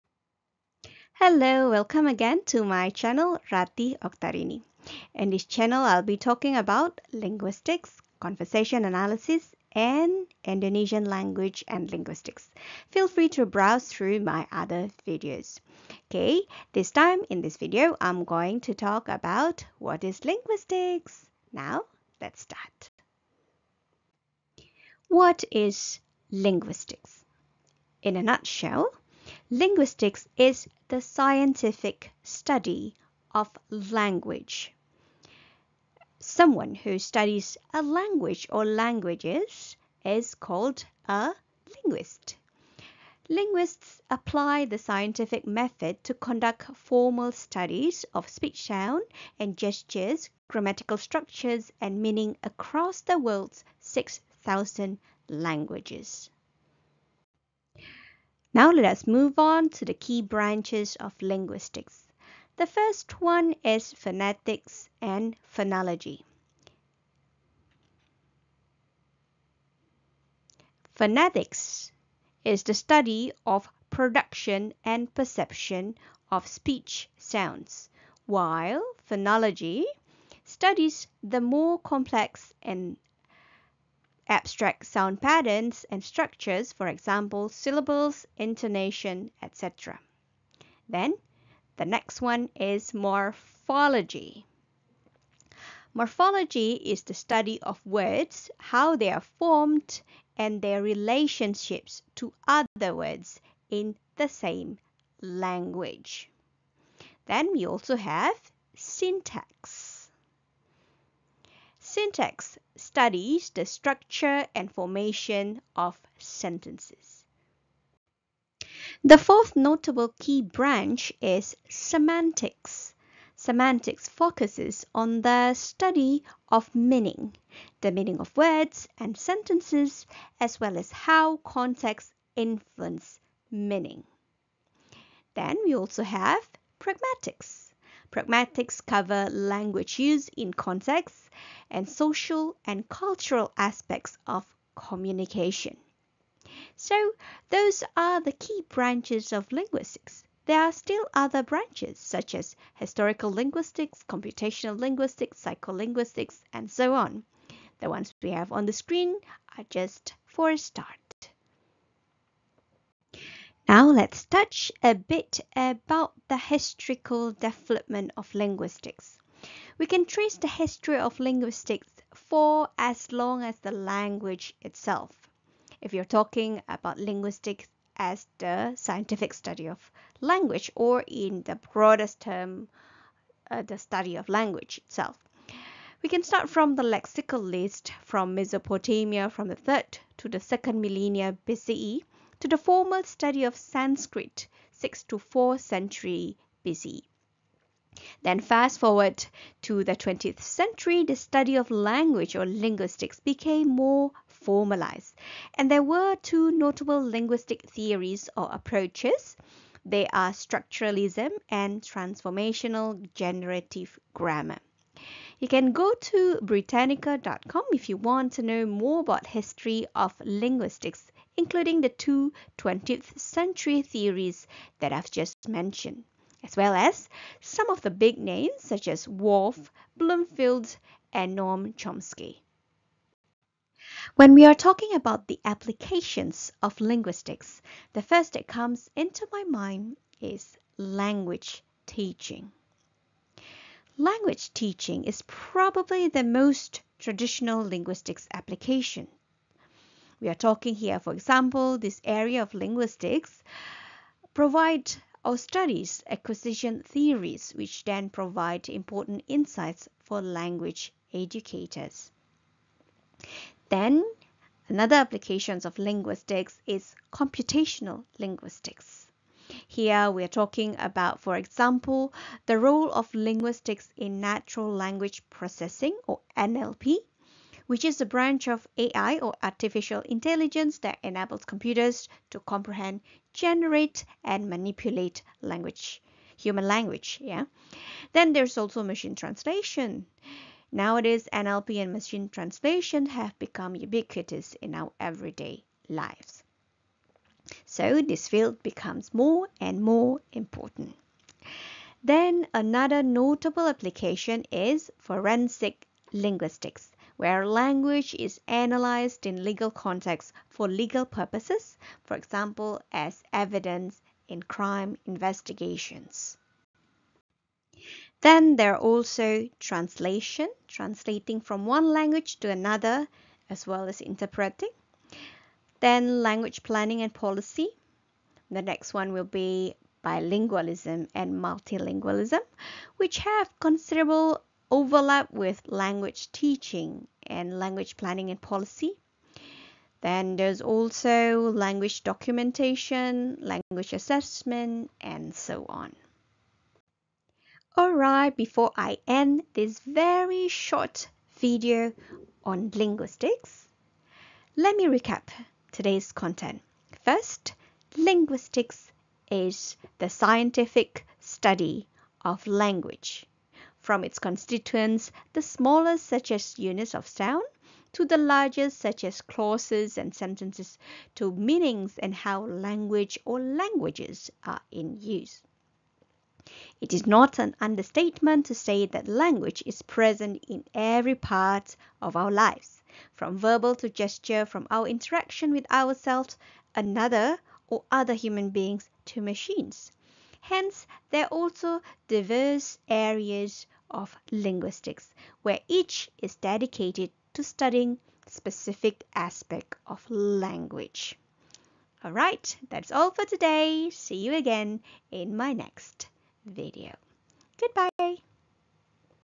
What is Linguistics?
What is Linguistics and its Key Areas

Chapters:
00:23 Definition of Linguistics
00:56 Key Branches of Linguistics
02:41 Historical Development of Linguistics
03:55 Applications of Linguistics
05:46 Conclusion: Recap of Linguistics